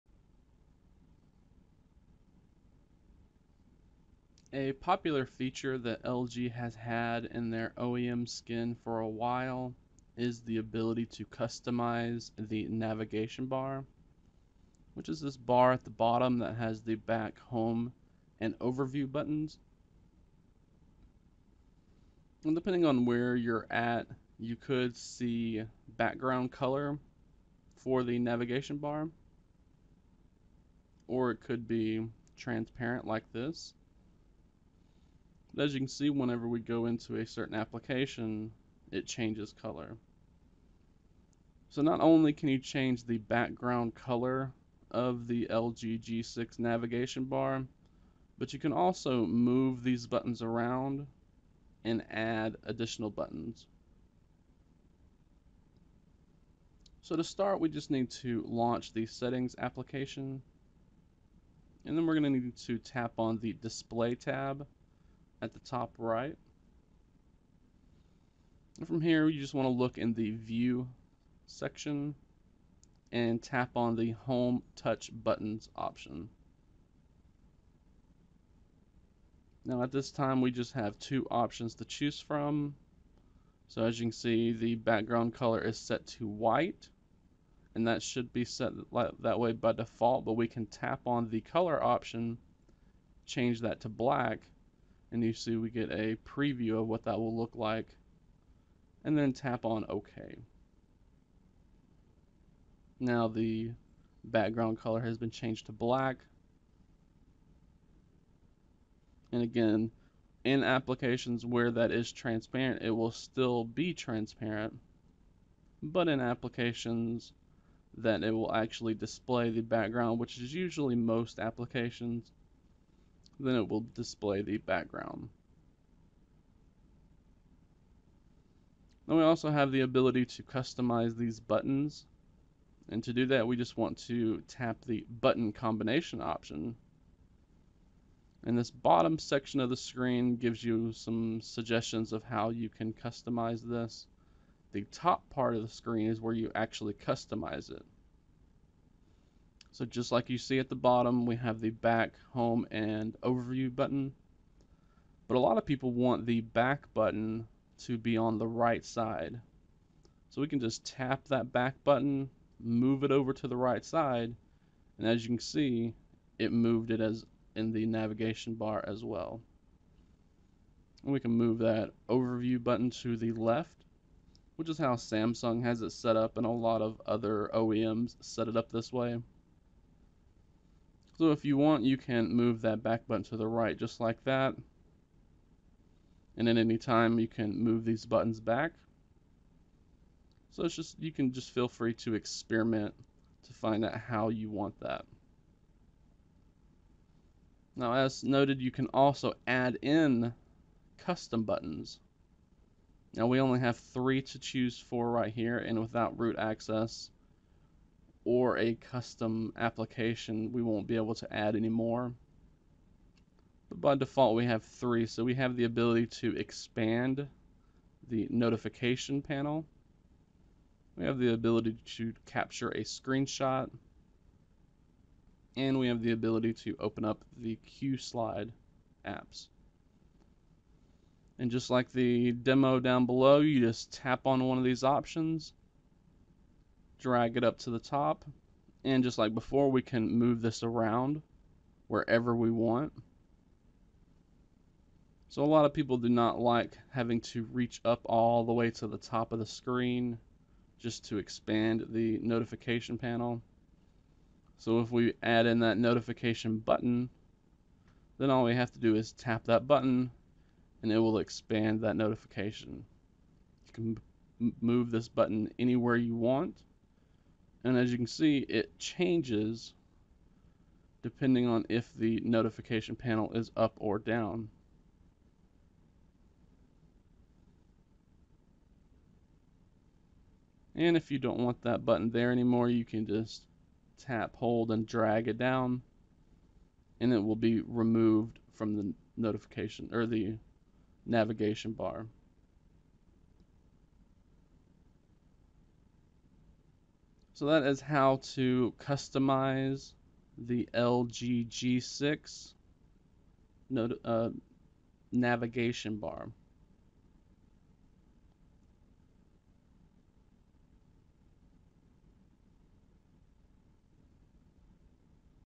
[LG G6] Customizing the Navigation Bar
Using LG's software, we can change both the color of the LG G6 Navigation Bar, as well as adding, moving and removing the buttons that are displayed there.